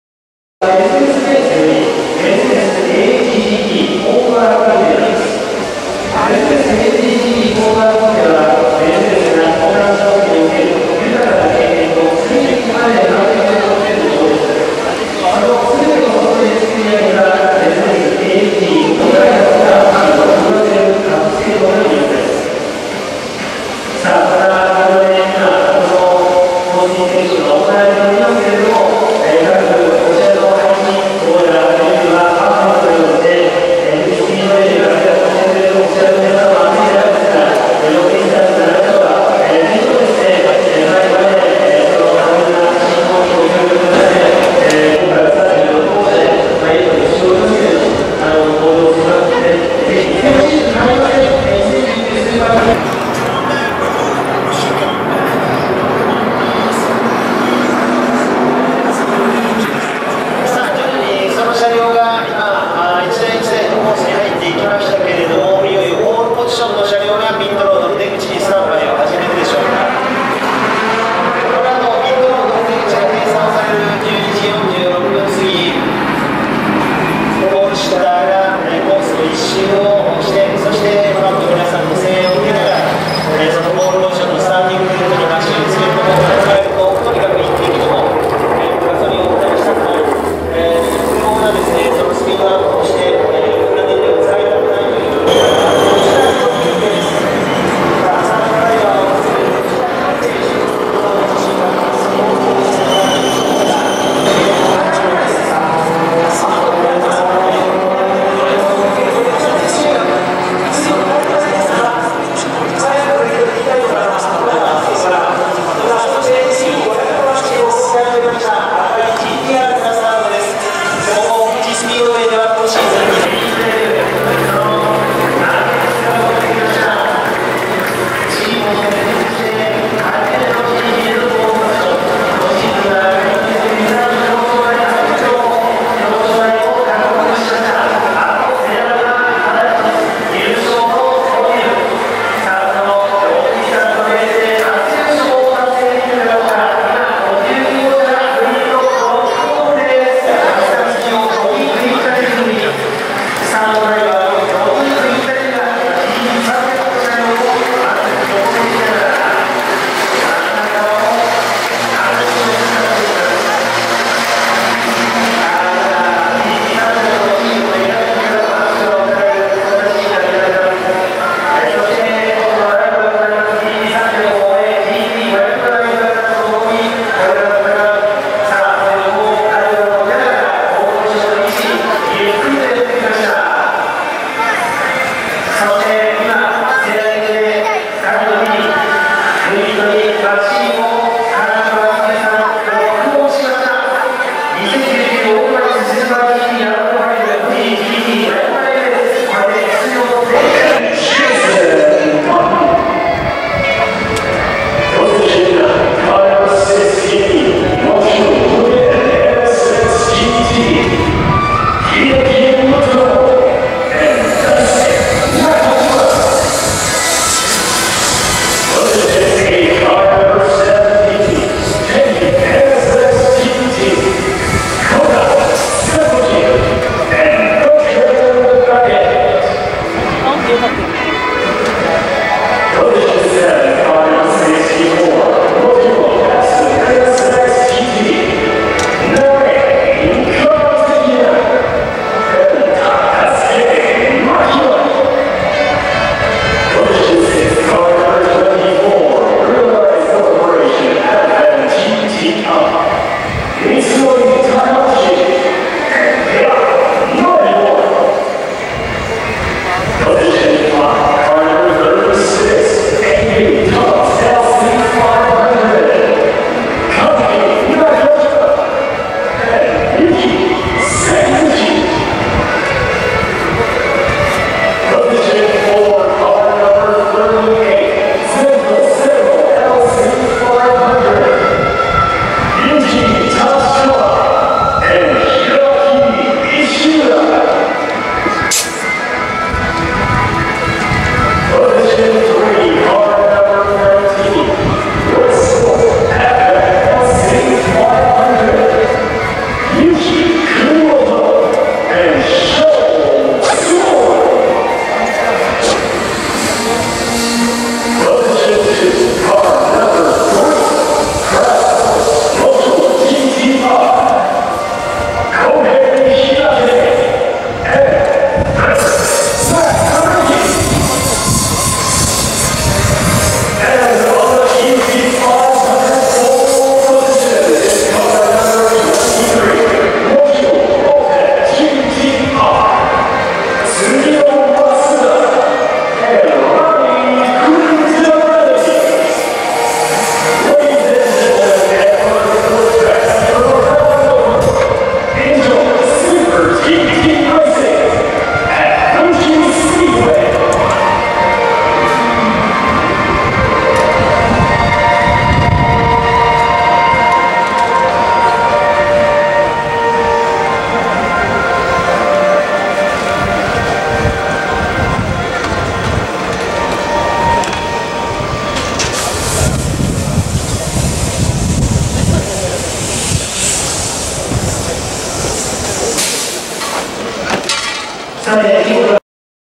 レースクイーン登場～GTマシンロックオン SuperGT富士2019 スタート進行
2019年8月3日（土）～8月4日（日）
FUJI GT500MILE RACE 富士スピードウェイ